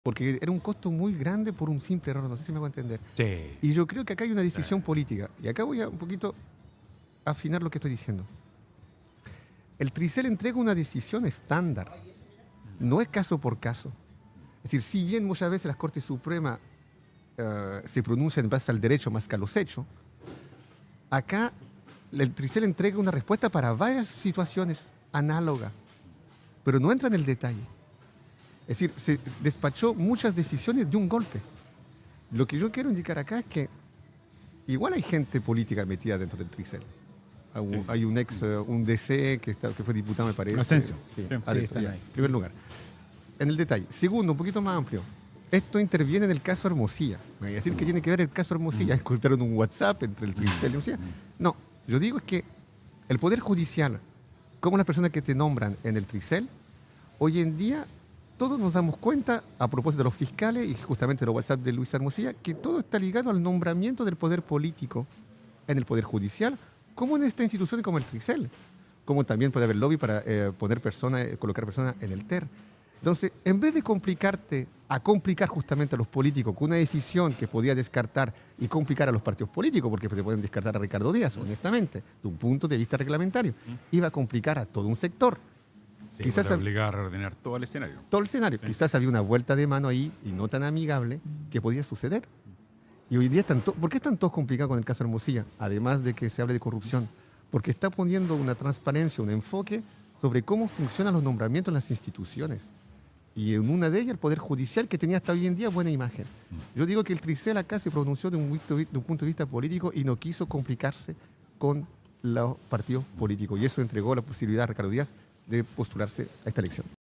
“El TRICEL se pronunció desde un punto de vista político (por el caso de Ricardo Díaz)”
CaféTL | “El TRICEL se pronunció desde un punto de vista político (por el caso de Ricardo Díaz)”

Cristian Zamorano, doctor en Ciencias Políticas